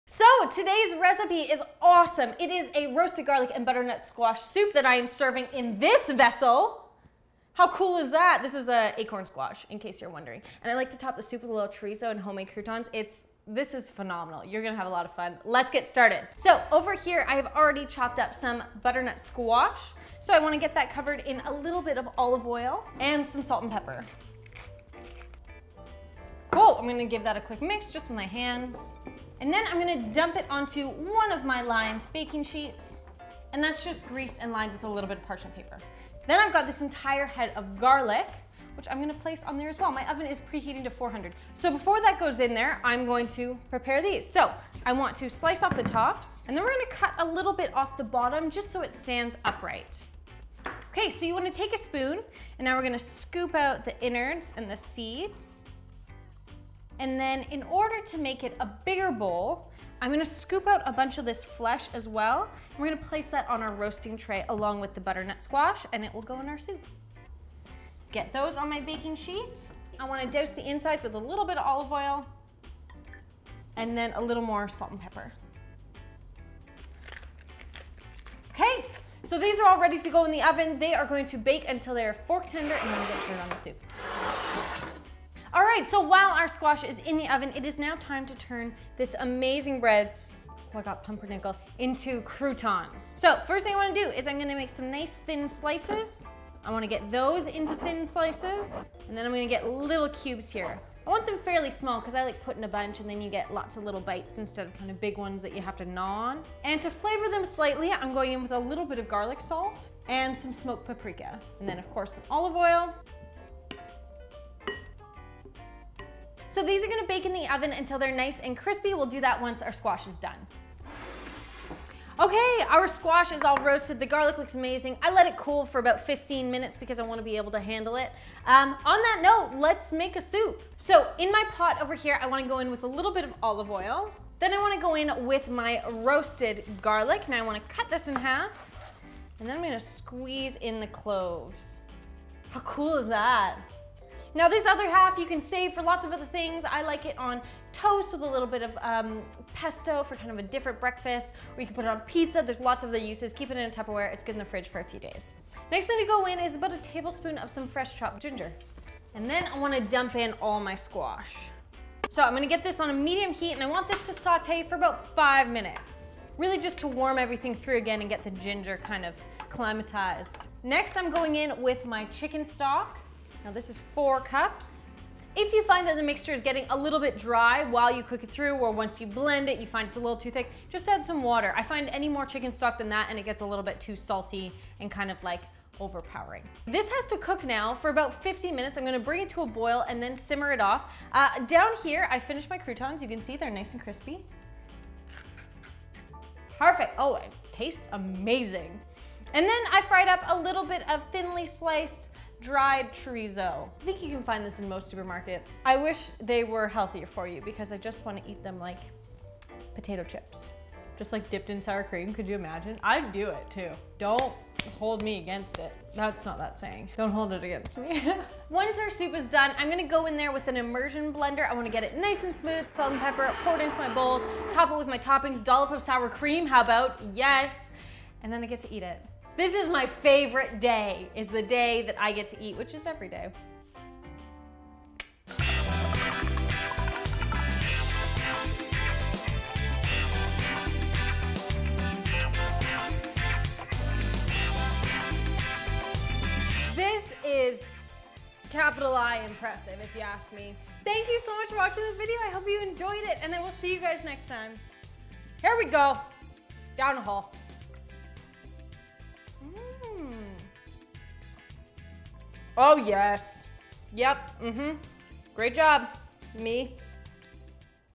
Squash Bowl Roasted Garlic Soup | The Tastemakers-Julie Nolke
What better way to serve butternut squash soup than inside an acorn squash?

Ingredients

2 Mini squashes or mini pumpkins as the bowls
1 head of garlic
1 acorn or butternut squash, seeded, peeled and chopped
Olive oil
1 tbsp fresh Ginger
Salt and Pepper
900ml Chicken Broth
2 Chorizo sausages
4 slices rye bread, cubed
1 tsp garlic salt
1 tsp smoked paprika
Sour cream

Steps

1.) Preheat your oven to 400˚F. On a baking sheet covered with parchment place the chopped squash and garlic head and drizzle with olive oil. Rub in with a little salt and pepper and bake for 30 minutes.
2.) To make the squash bowls, slice off the stem lid of the squash and slice off a small portion of the bottom so it stands straight. Remove the seeds and flesh and place on a baking sheet and roast until tender, 20 – 30 minutes.
3.) Once the squash is baked and fork tender, place it in a large pot along with some olive oil, the roasted garlic and ginger. Sauté on high for 5 minutes. Add in the chicken broth and a little salt and pepper. Bring to boil and then reduce down to a simmer for 15 minutes. In the meantime, fry up the chorizo and prepare the croutons by coating the bread in oil, paprika and garlic salt and baking at 350˚ until crispy.
4.) Using an immersion blender, blend the soup until smooth. To serve, ladle the soup into each squash bowl and top with croutons, thin slices of the sausages, and a dollop of sour cream.